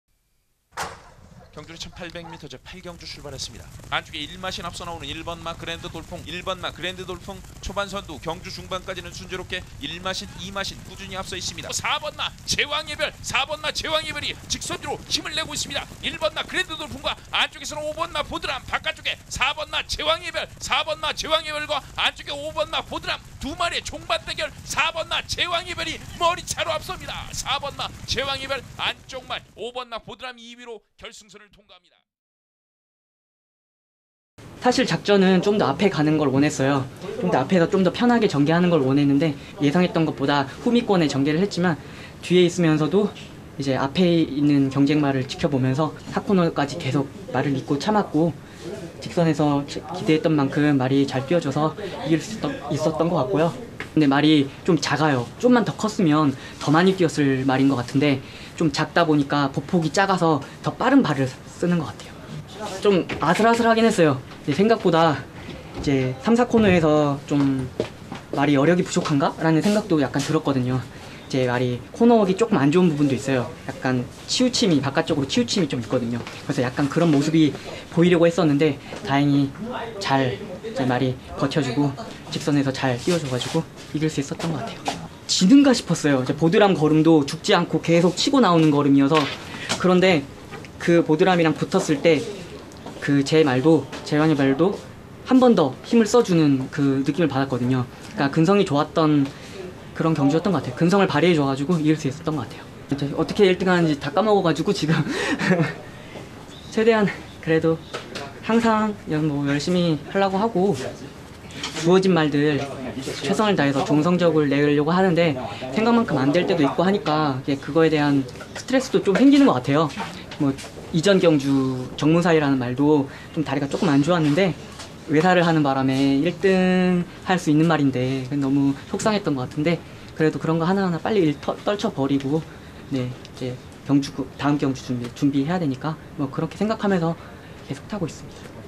6월 29일 오늘의 하태핫태! (서울8경주 김동수)
6월 29일 오늘의 하태핫태!
서울8경주 김동수 기수 & 제왕의별 우승!